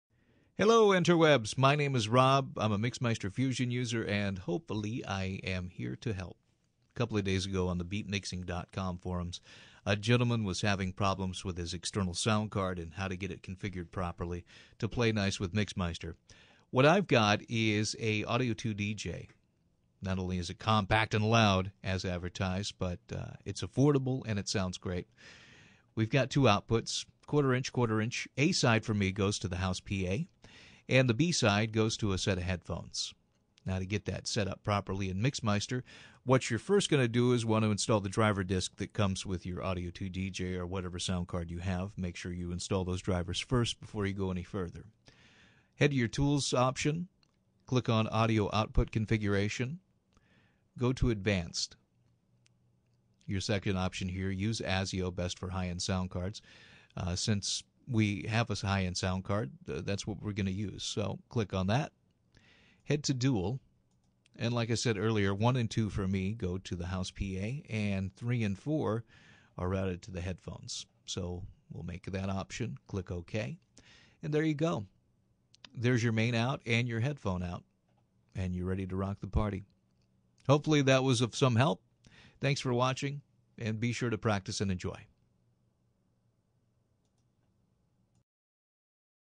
Mixmeister Fusion Tutorial (external soundcard)
how to configure external soundcard to work with mixmeister